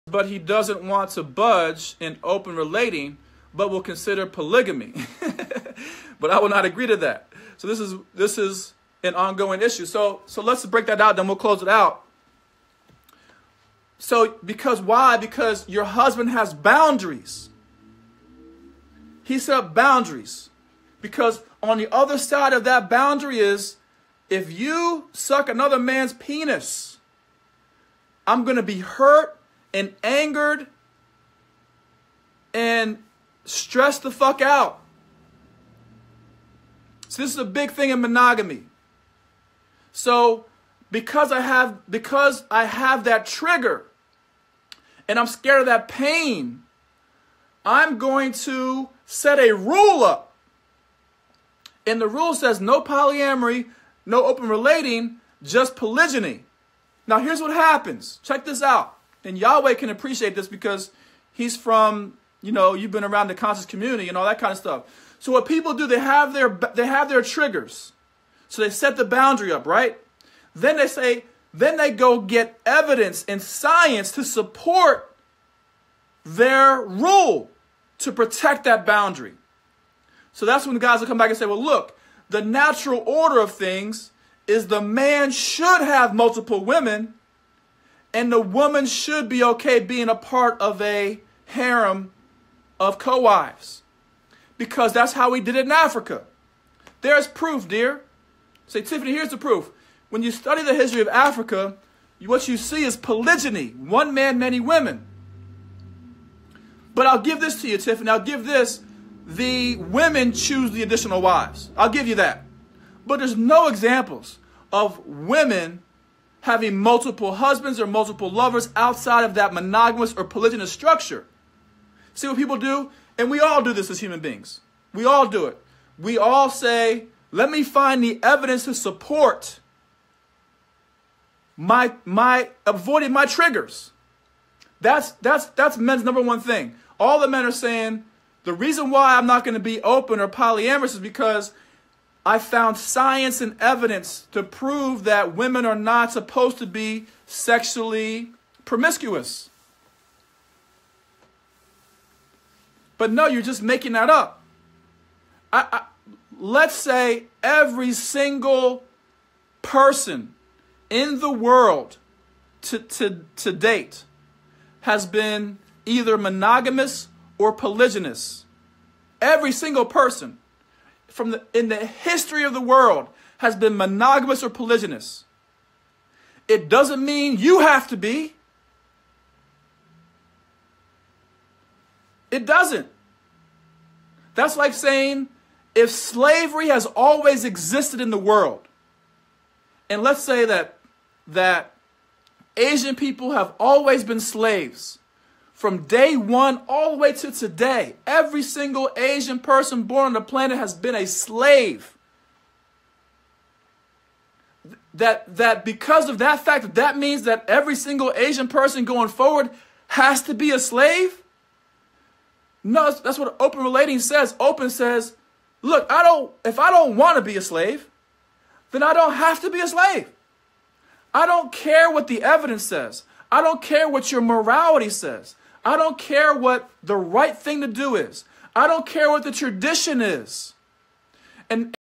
Why Men Want Polygyny & How They Justify It
This is a clip from my Facebook Live about Open Relating.  We covered:
--Agreements
--Rules
--Expectations
--Contracts
--Responsibility
--Boundaries
--Standards

See the full lecture in the JLA Resource Library.  (for men)